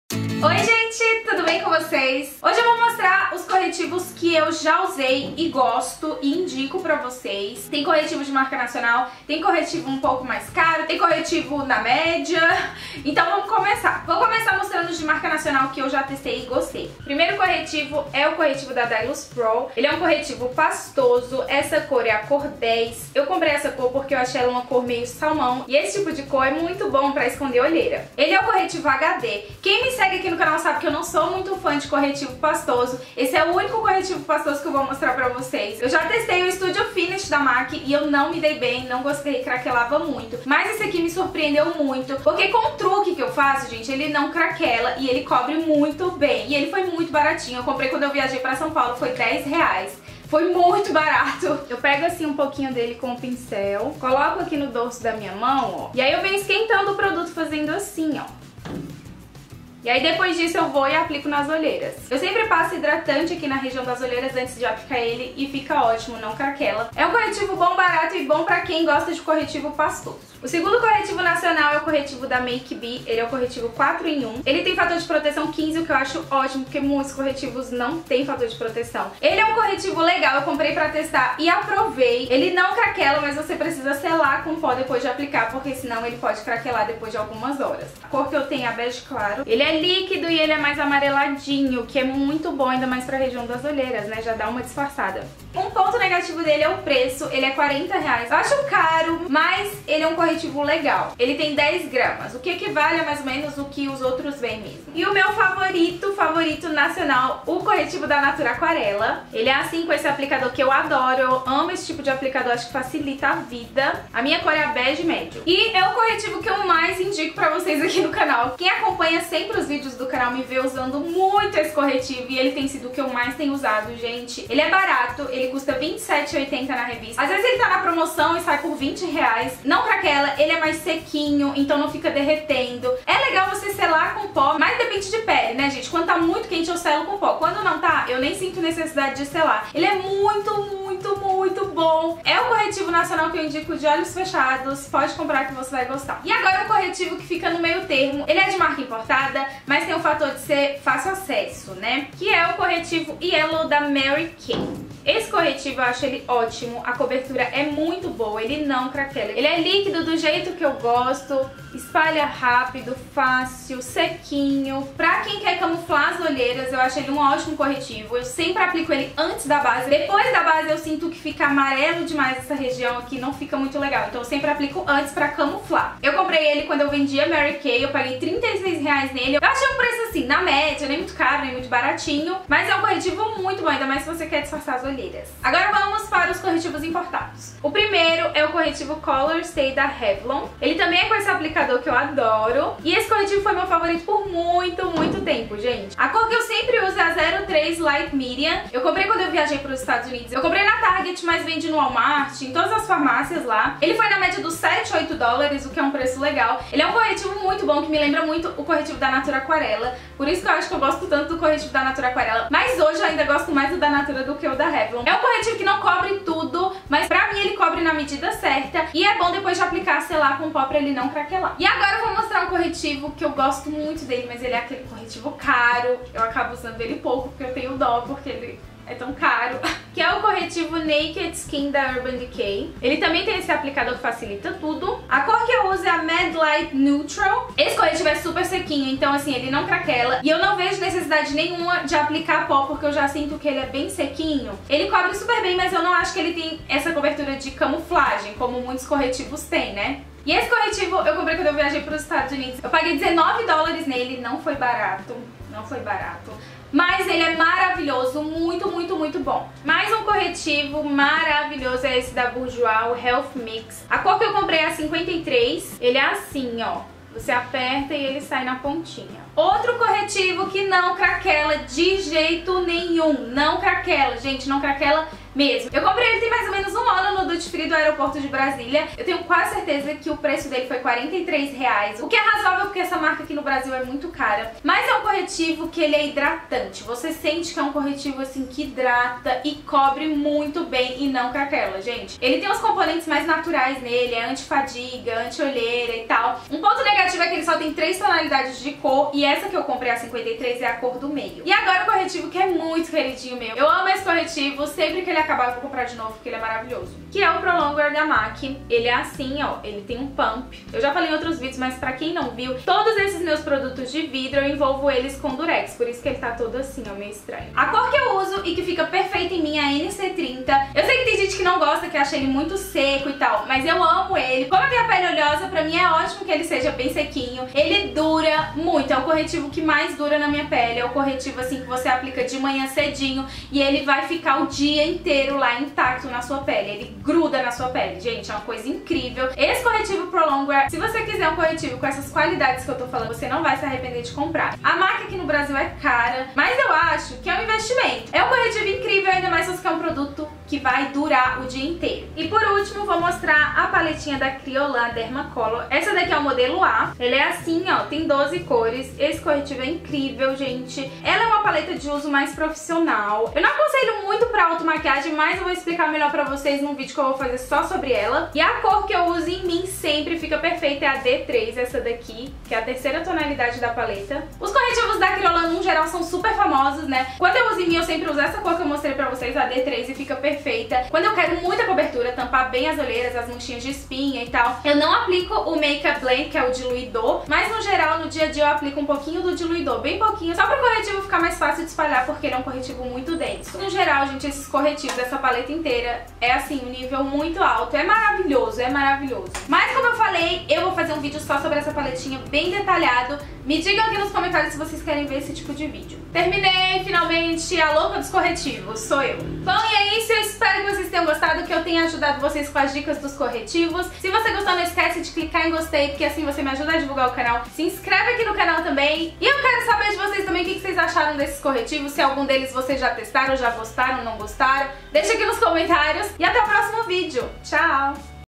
Corretivos Favoritos da Vida
Oi, gentee!!!

Hoje vim passar dicas de todos os corretivos que eu já usei e super aprovei! Vem conferir! ;D

Batom que estou usando: Cruella - Velvet Matte Lip Pencil - Nars

Produtos:
Corretivo HD Dailus Pro cor 06
Corretivo 4 em 1 cor bege claro - Make B.
Corretivo Natura Aquarela cor bege médio
Corretivo Yellow - Mary Kay
Corretivo Color Stay cor 03 Light Medium - Revlon
Corretivo Naked Skin cor Med Light Neutral - Urban Decay
Corretivo Healthy Mix cor 53 - Bourjois
Corretivo Pro LongWear cor NC30 - M.A.C
Paleta modelo "A" Kryolan DermaColor

Caixa Postal: Nº 2401| CEP: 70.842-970 - Brasília/DF